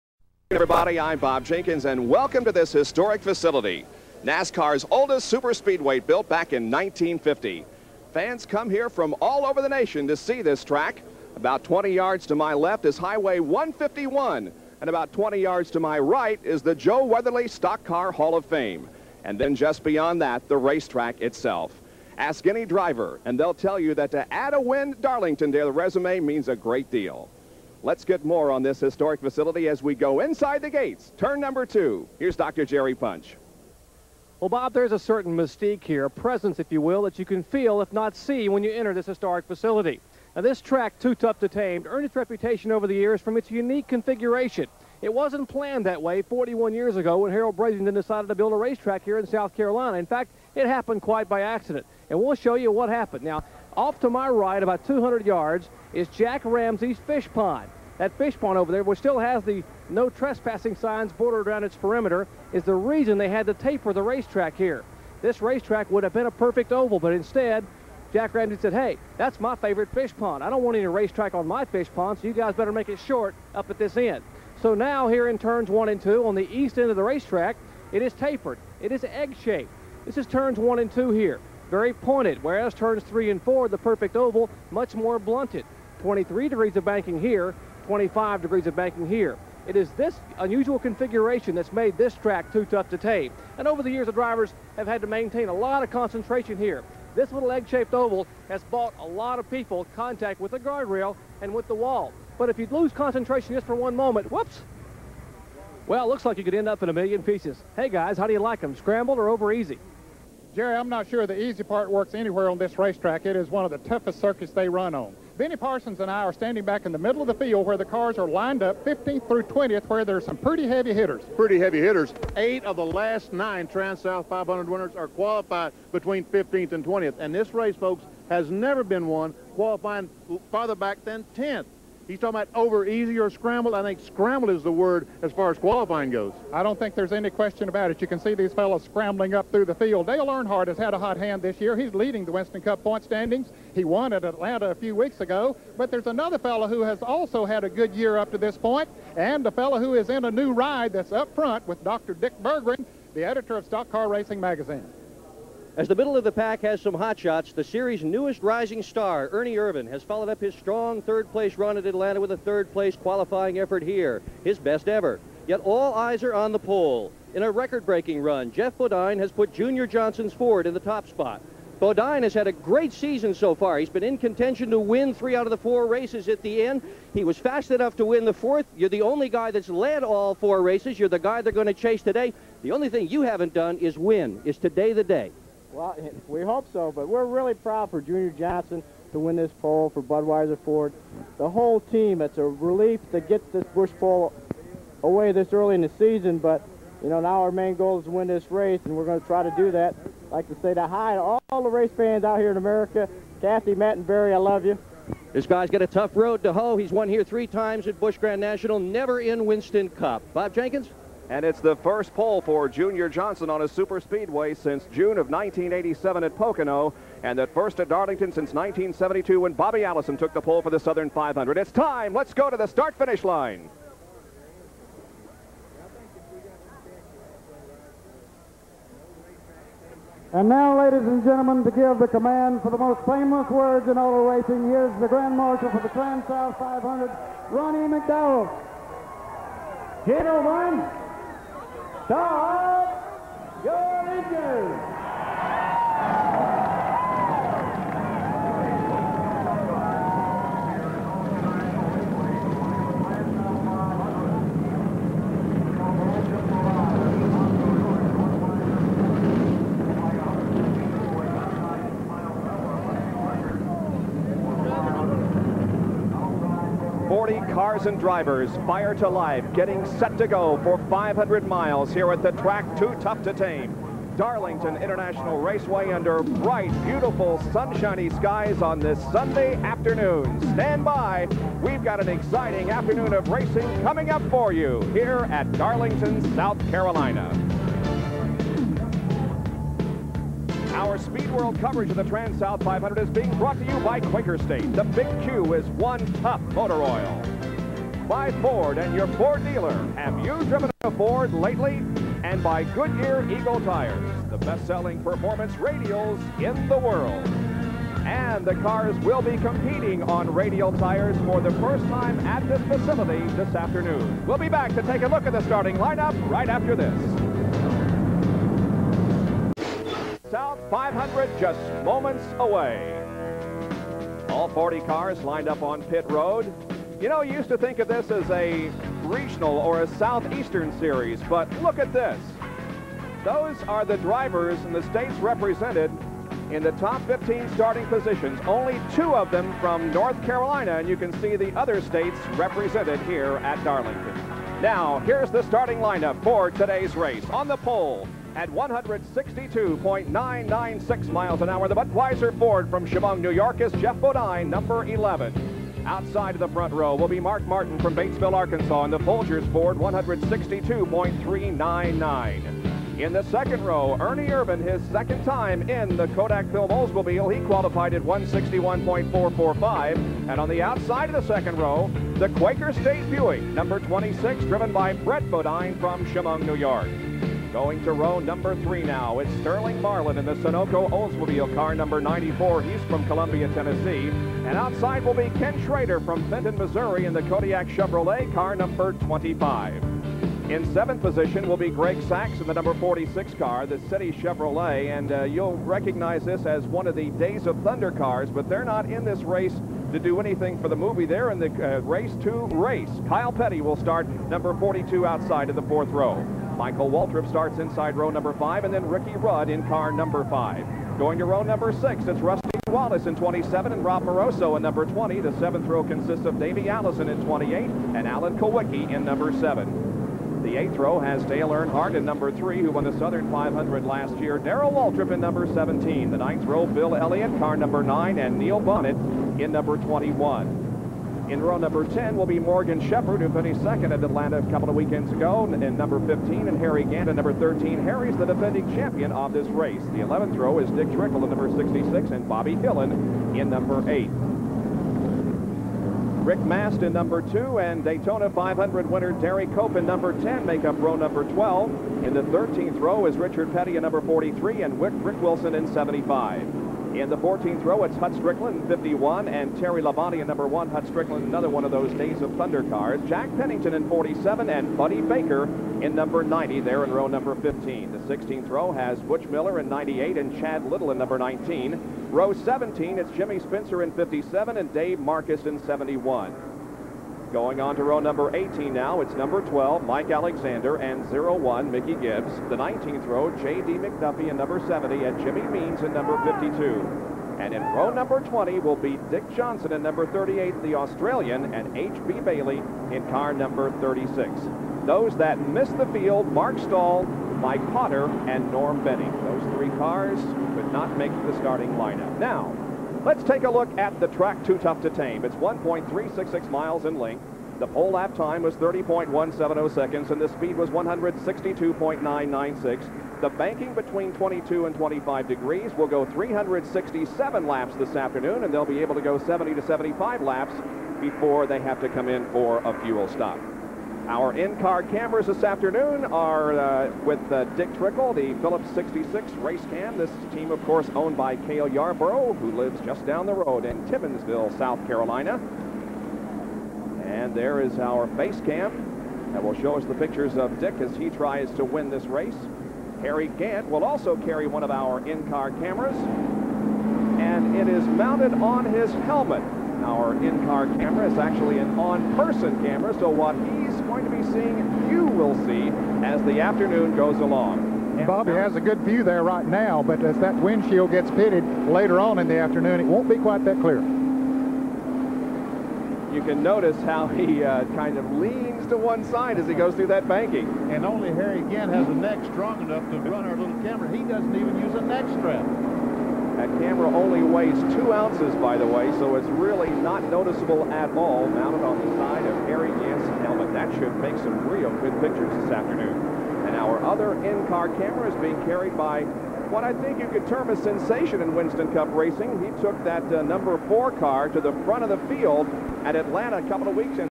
1990 Transouth 500 - PART 1/20 (Intro/Grid)
This is a race I've been getting requests for, for a while now.
We go back to 1990 (Cant believe it was 20 years ago) as the series hits the Lady In Black for the Transouth 500 at Darlington! Dale Earnhardt is riding momentum from a win at Atlanta while there is this new young hot shot named Ernie Irvan taking the series by storm. Not too mention the filming of Days Of Thunder is in full swing as the Movie cars are going for it!

*Part 1 quality is a little iffy but it settles down by part 2*

1990 Transouth 500
Darlington Raceway